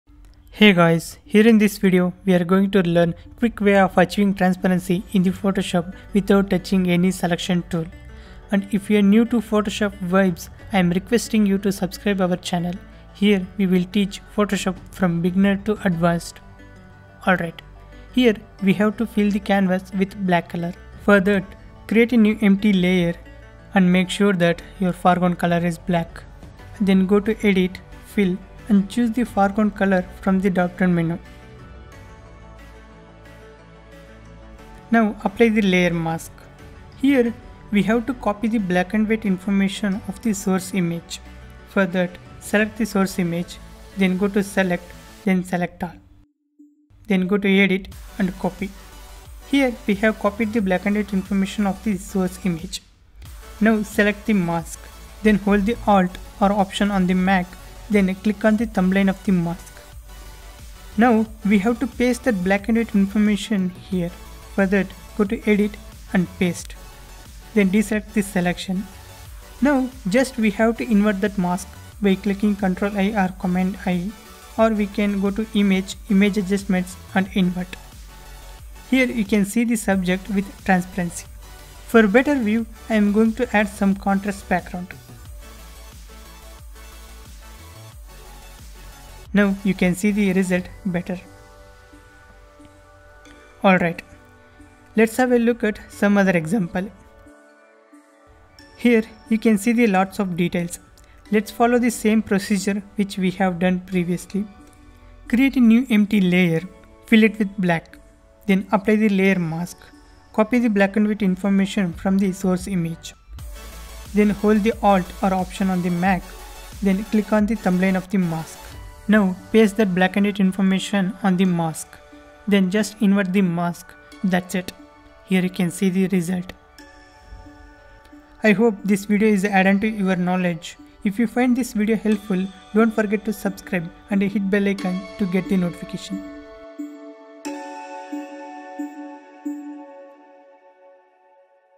Create transparent background just in 10 seconds | Photoshop vibes
Here in this video, we are going to learn Quick Way of Achieving Transparency in Photoshop